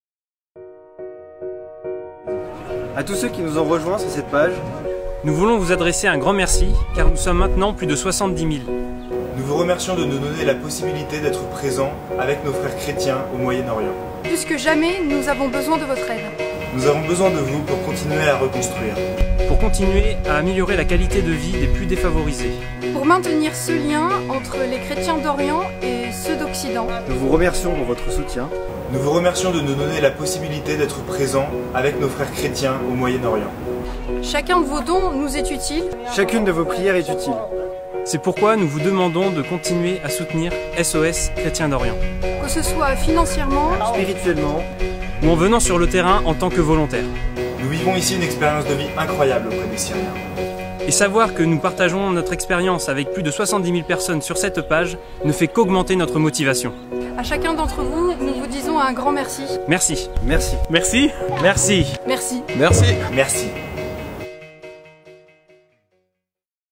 [Syrie] 70 000 !!!
Vous êtes maintenant plus de 70 000 personnes à suivre les volontaires sur le terrain via la page facebook de SOS. Nous voulions vous adresser un grand MERCI pour votre soutien.

Votre aide nous est très précieuse pour mener à bien les actions auprès des chrétiens d'Orient.

[SYRIA 🇸🇾] 70,000 !!!

You are now more than 70,000 people to follow the volunteers on the ground via this facebook page. We wanted to THANK YOU all for your inconditionnal support.

Your help is very important to carry out the actions for the Christians of the East.

[СИРИЯ 🇸🇾] 70,000 !!!

Мы хотели сказать вам большое СПАСИБО за вашу поддержку.

Ваша помощь очень ценна для того, чтобы успешно провести наши проекты среди христиан Востока.

ВСЕМ БОЛЬШОЕ СПАСИБО!